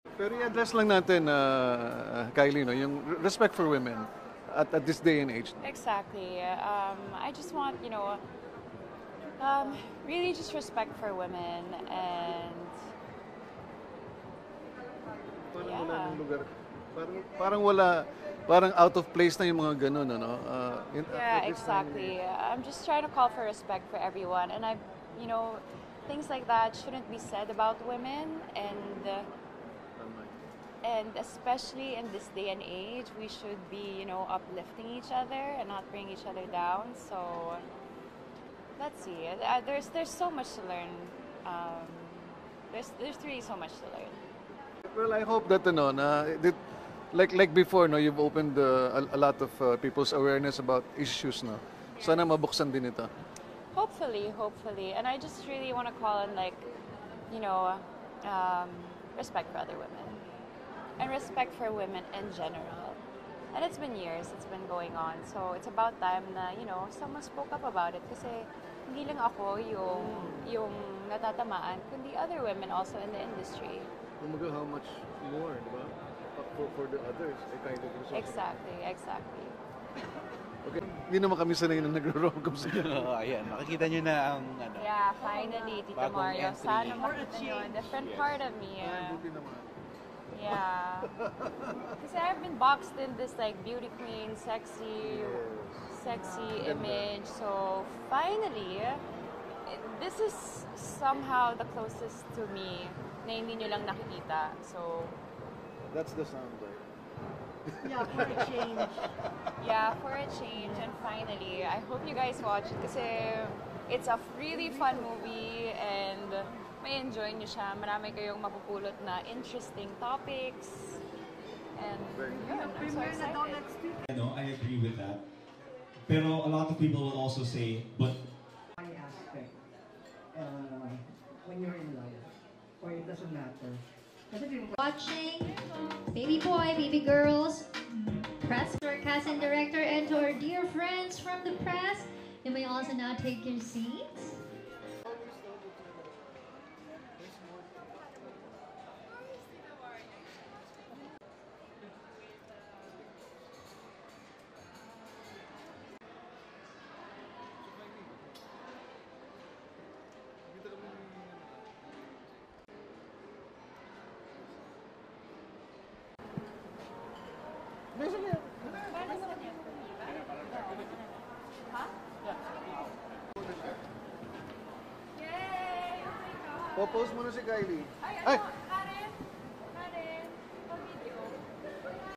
Lovely Kylie Verzosa reacts to “mukhang tuyot” write-up
Beauty queen-actress Kylie Verzosa expressed dismay at a recent online article-headline describing her as looking “tuyot at retokada”. “Let’s respect all women, things like that shouldn’t be said about women in this day and age. This has been going on for years, di lang ako ang tinatamaan. It’s about time someone spoke about it,”  said Kylie at the sidelines of her latest project, the sexy romance-comedy “Baby Boy, Baby Girl” with Marco Gumabao and directed by JP Laxamana. Kylie also pegged her amusing portrayal of an escort for hire in the movie as milestone in her movie career. “ I’ve been boxed into this beauty queen sexy star image so finally for a change, makikita niyo na ako in a different light!”

(Video Courtesy: ABS-CBN News)